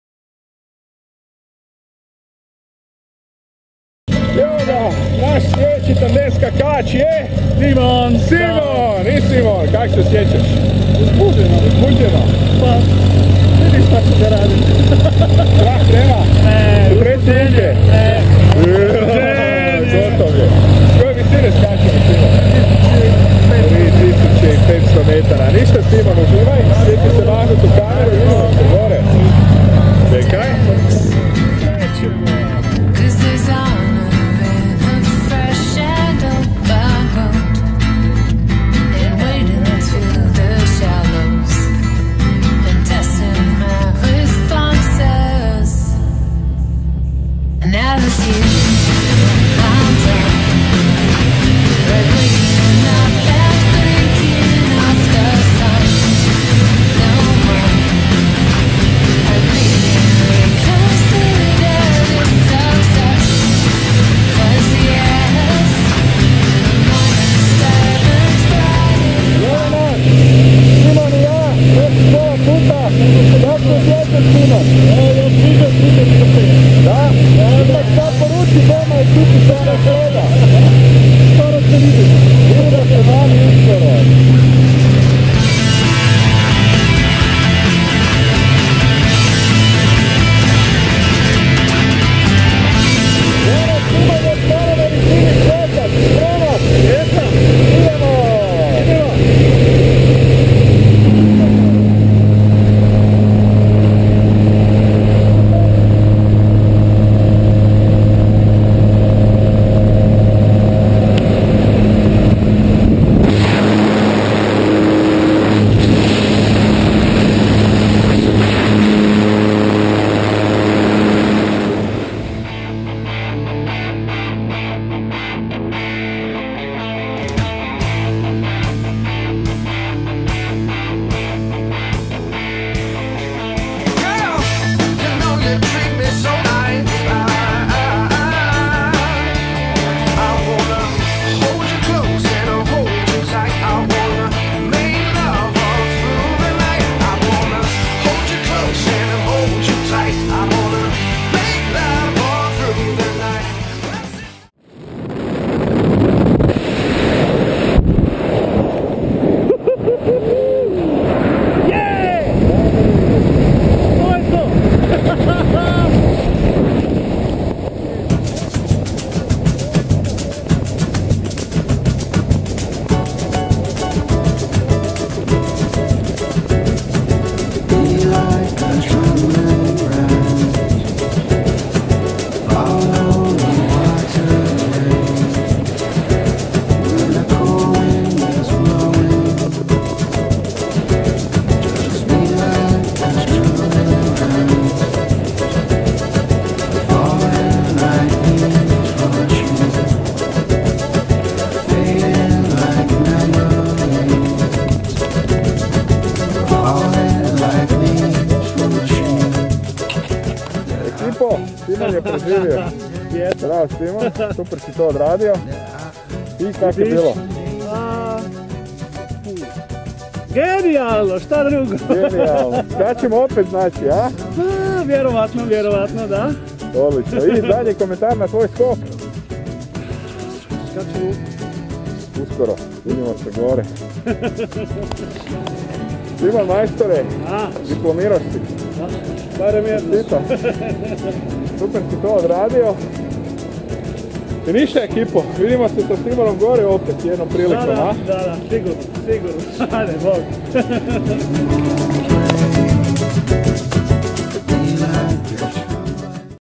Tandem Skok - Simon Razloznik - Padobranski Sportski Klub Slobodan Pad
Padobranski Sportski Klub Slobodan Pad - Tandem skok - Simon Razloznik
Tandem padobranski skok, skok padobranom iz aviona, padobranstvo ili slobodan pad - za one koji nikad nisu iskusili čari padobranstva i slobodnog pada brzi vodič kroz vaš prvi tandem padobranski skok iz aviona